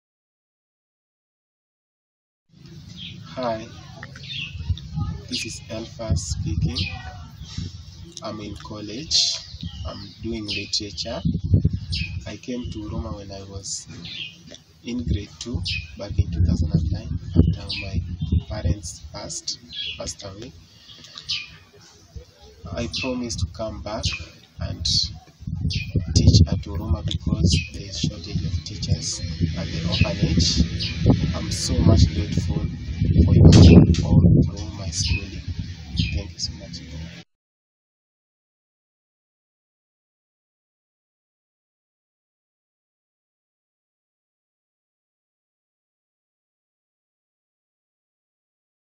Elphas Studies Literature and International Tourism
Elphas is in his third year of University. He is doing very well.